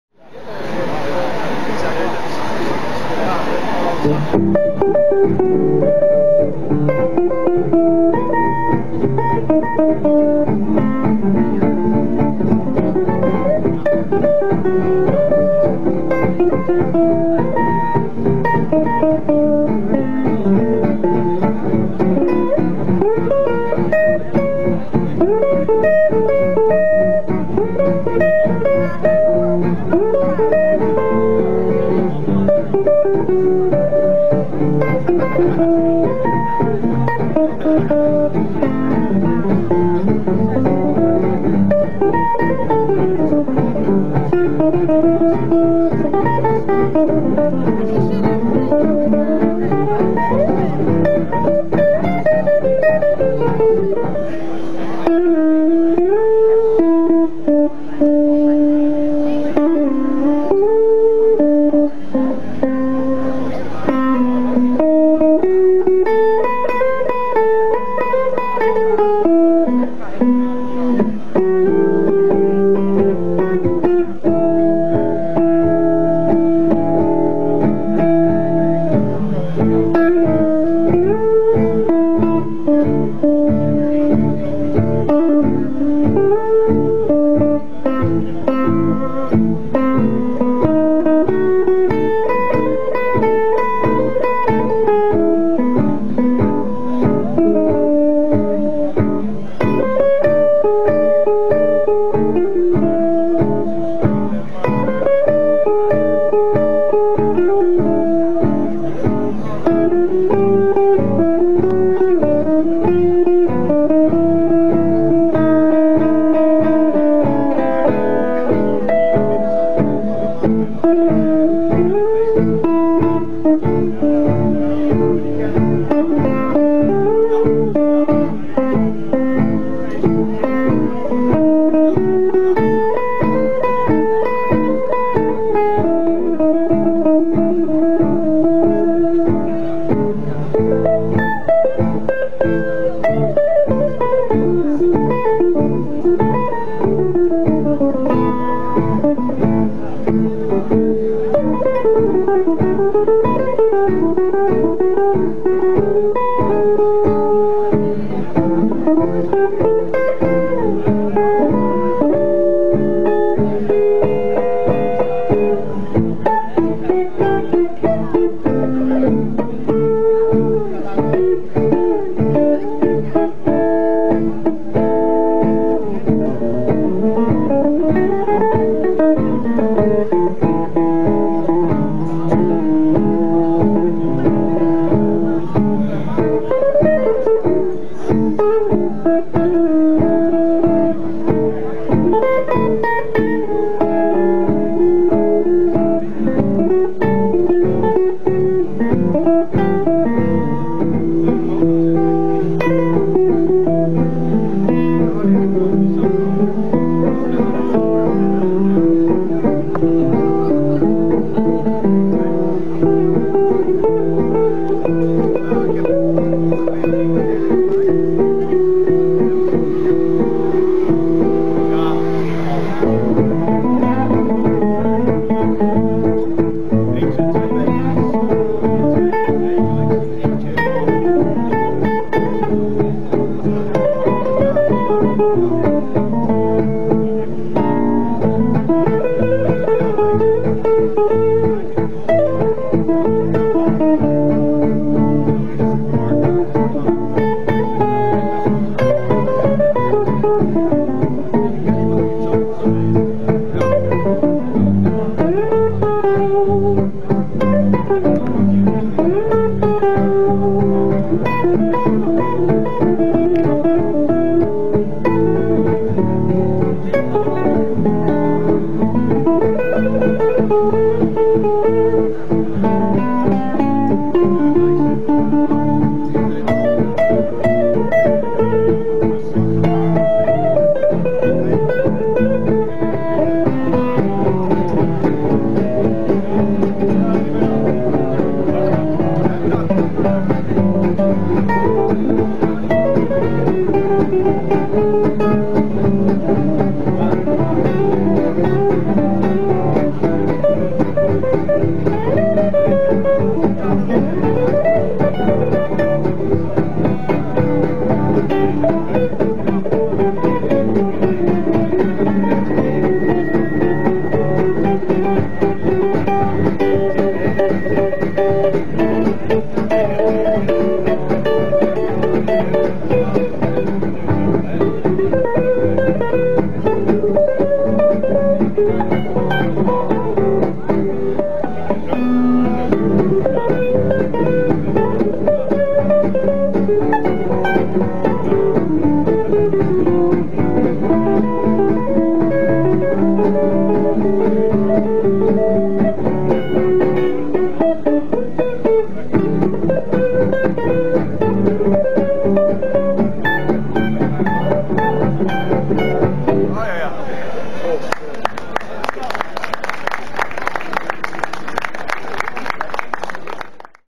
Django Reinhardt festival Tilburg 2014
Fragments of our little gig in Tilburg with some of my own gypsyjazz songs. With my good friend Mimmo Guarragi.